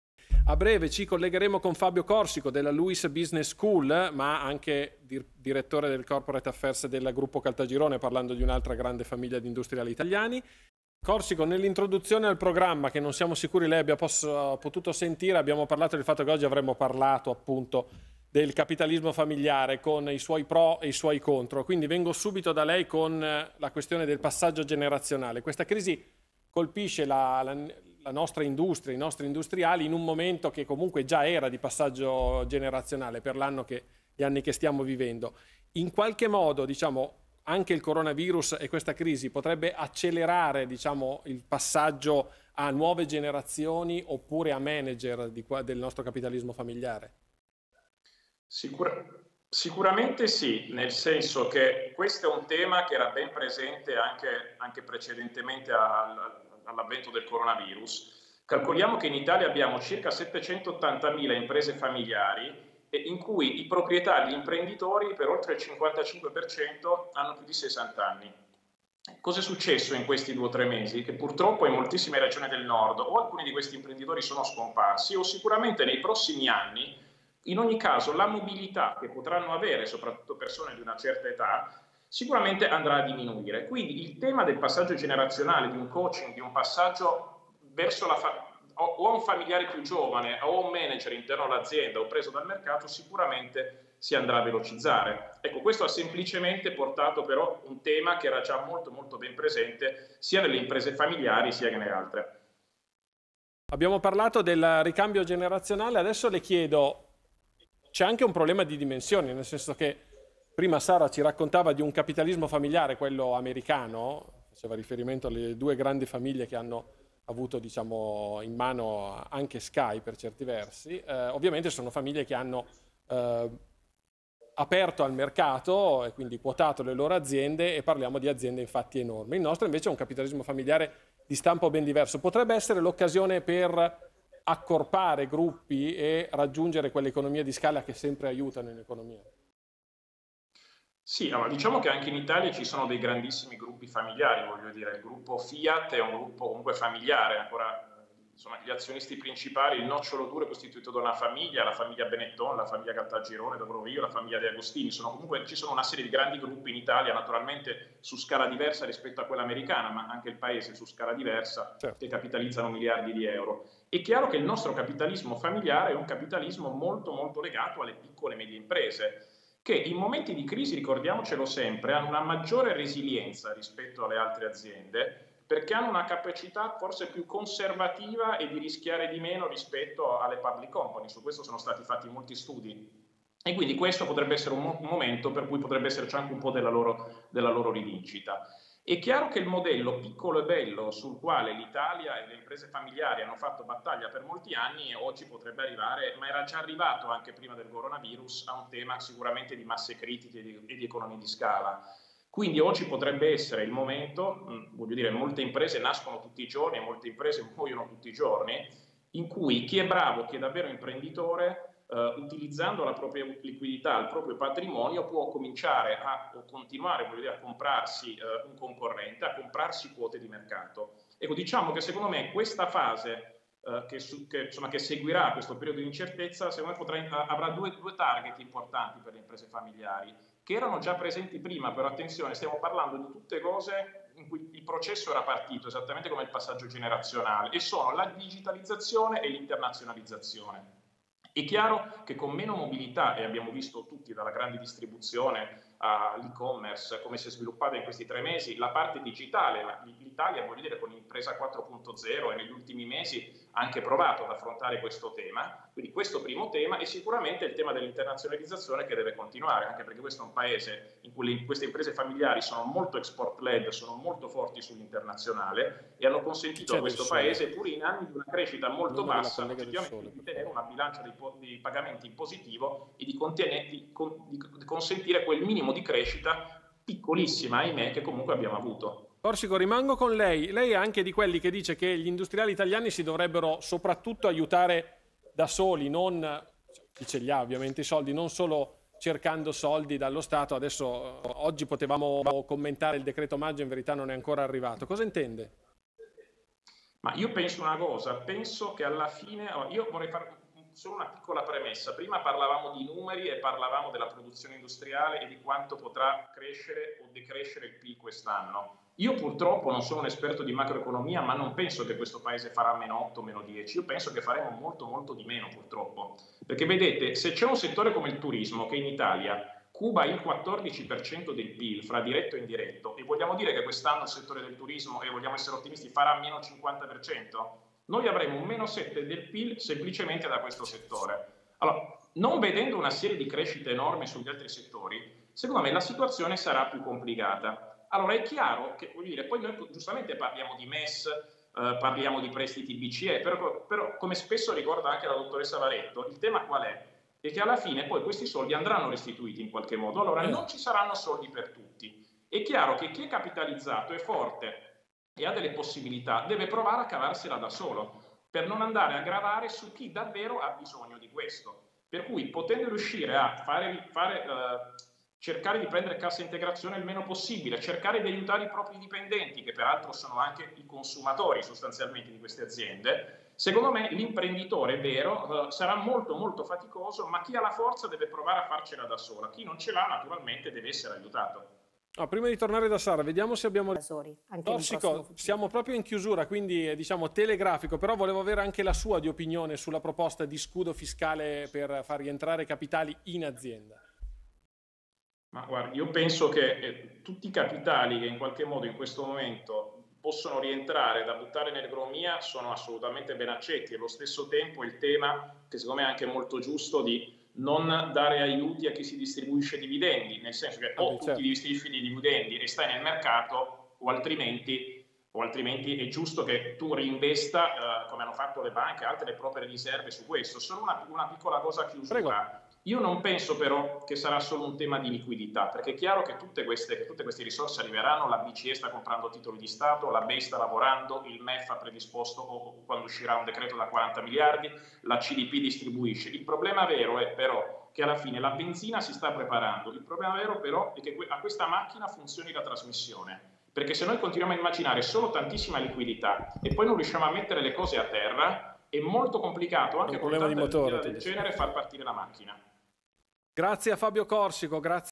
Imprese familiari, Covid-19 e passaggio generazionale
L'emergenza Covid-19 e il passaggio generazionale nelle imprese familiari: l'intervento di Fabio Corsico, Direttore Executive Programme in Global Family Business Management, a SKY TG24.

// LINKS

// MAIL

Luiss Business School
Villa Blanc, Via Nomentana, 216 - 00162 Roma
Roma, Italia